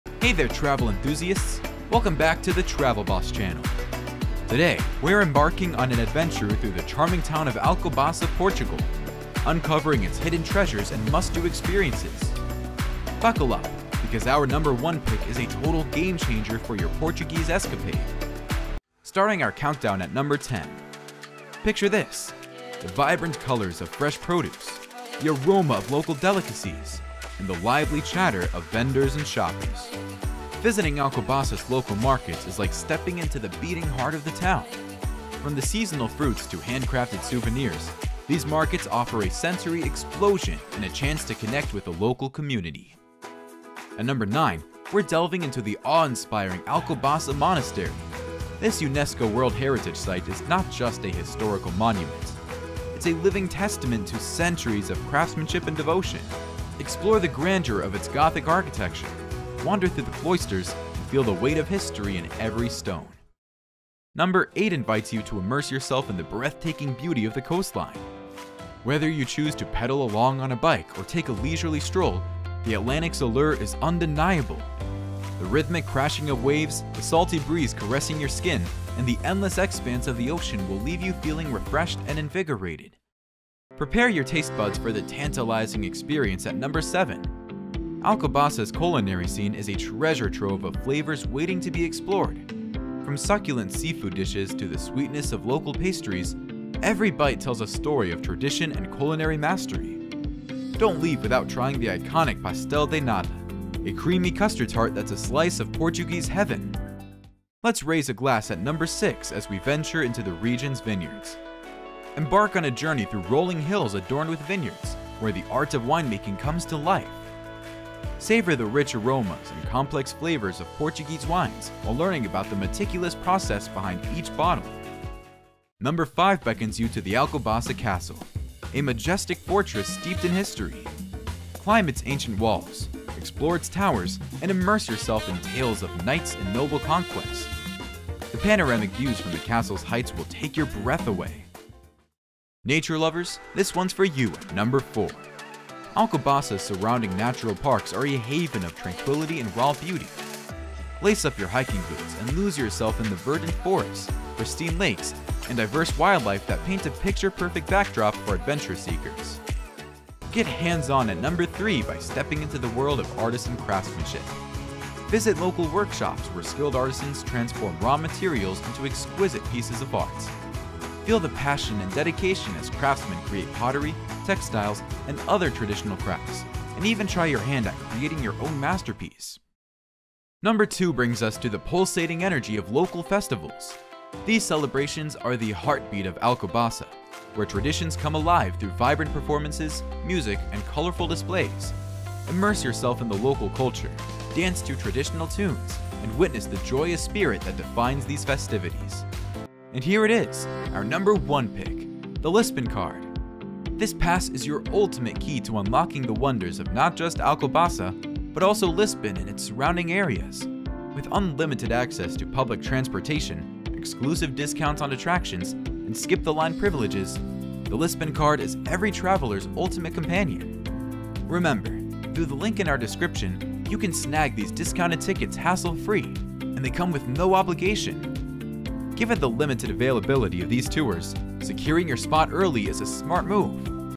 TOP 10 Things to do in Alcobaca, Portugal 2024!
Alcobaca, Portugal is an amazing place to visit in Portugal. If you want to know top 10 what to do in Alcobaca or you need a travel guide, please keep watching.

Nature lovers, this one's for you at number 4! Alcobaca's surrounding natural parks are a haven of tranquility and raw beauty. Lace up your hiking boots and lose yourself in the verdant forests, pristine lakes, and diverse wildlife that paint a picture-perfect backdrop for adventure seekers.

Get hands-on at number 3 by stepping into the world of artisan craftsmanship. Visit local workshops where skilled artisans transform raw materials into exquisite pieces of art. Feel the passion and dedication as craftsmen create pottery, textiles, and other traditional crafts, and even try your hand at creating your own masterpiece.

Number 2 brings us the pulsating energy of local festivals. These celebrations are the heartbeat of Alcobaca, where traditions come alive through vibrant performances, music, and colorful displays. Immerse yourself in the local culture, dance to traditional tunes, and witness the joyous spirit that defines these festivities.

And here it is, our number one pick, the Lisbon Card! This pass is your ultimate key to unlocking the wonders of not just Alcobaca but also Lisbon and its surrounding areas. With unlimited access to public transportation, exclusive discounts on attractions, and skip-the-line privileges, the Lisbon Card is every traveler's ultimate companion.